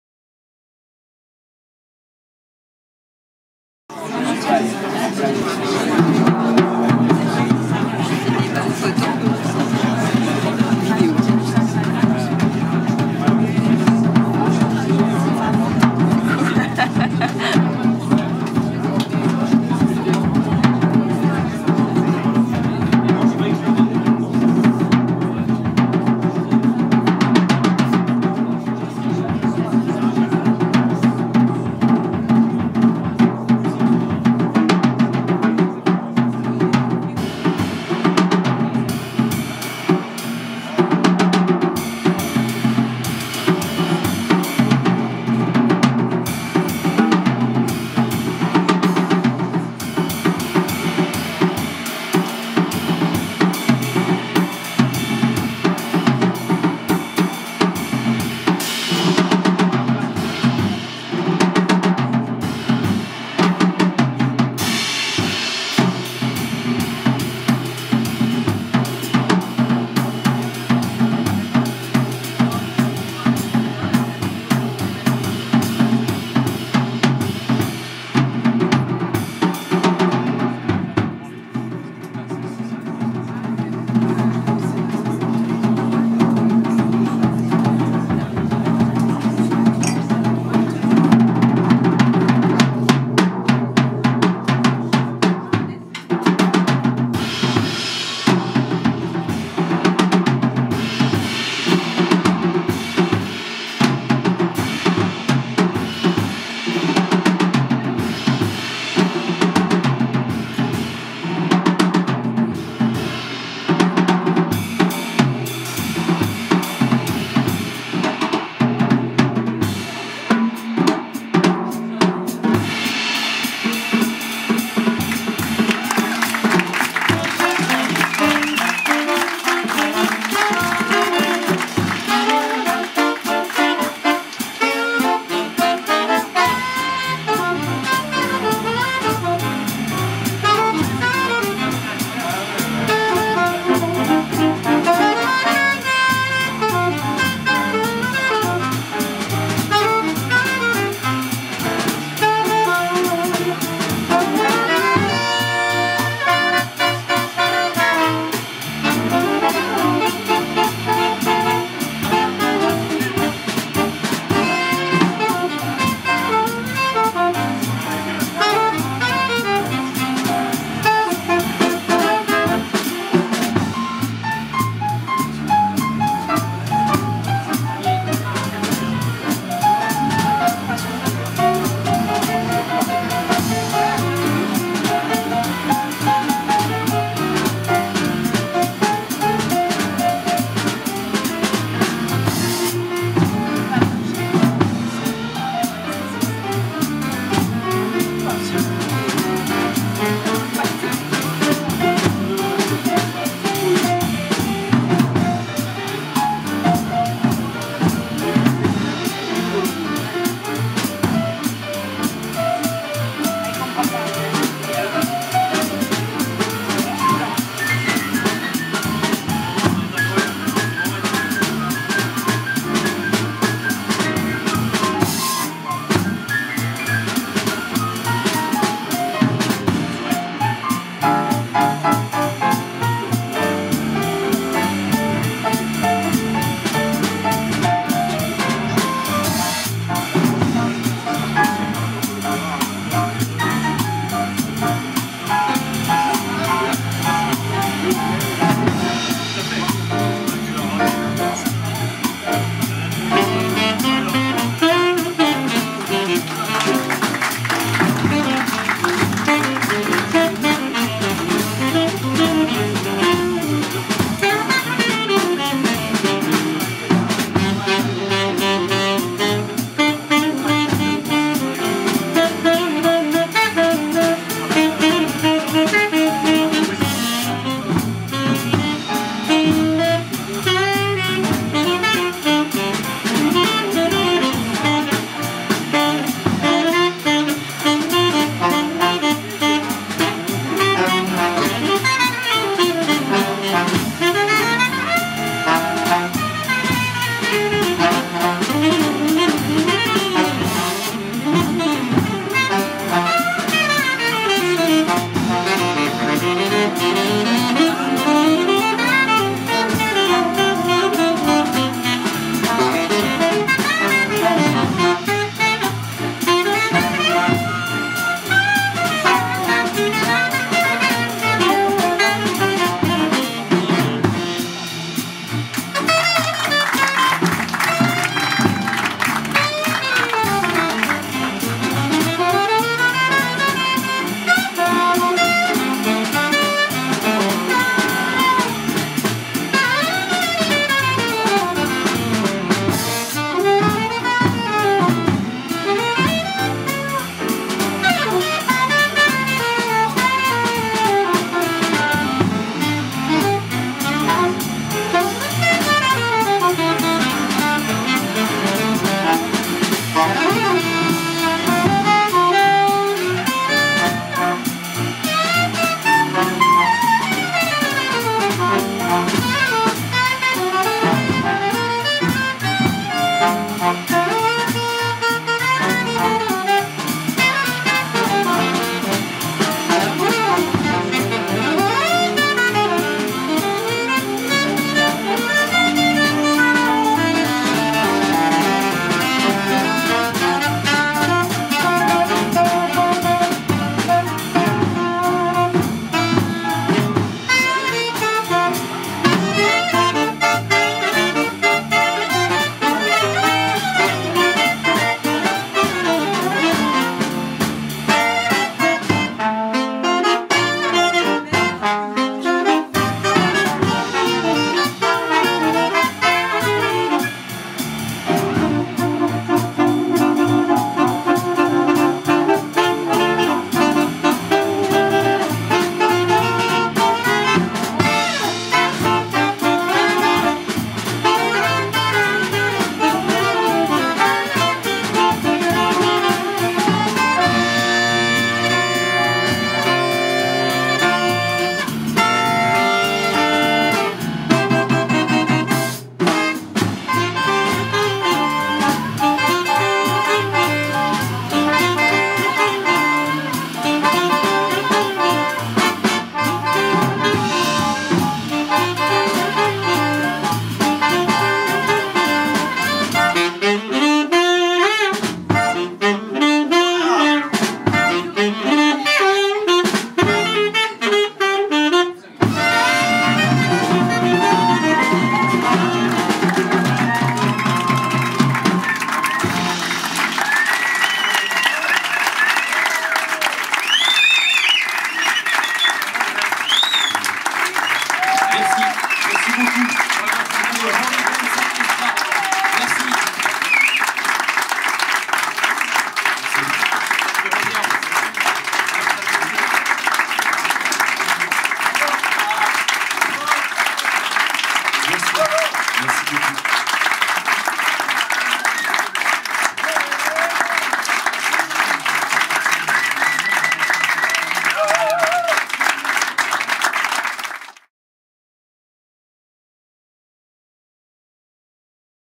HOLY BOUNCE ORCHESTRA, La Clef de Voute, Lyon,par la Photographe Auteure Fabienne Alliou Lucas
Film réalisé par la Photographe Auteure Fabienne Alliou-Lucas, Lyon, 2019, Concert